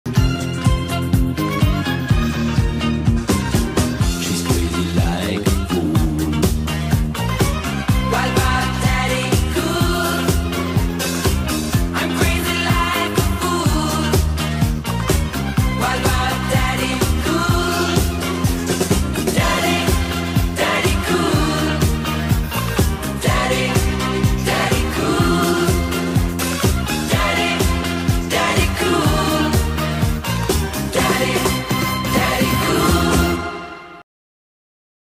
Boney M - Mayfest 2023 | Rixos Premium Seagate
The greatest music, legendary hits of the 70s, and positive vibes only - it all describes @boneym.official  performance.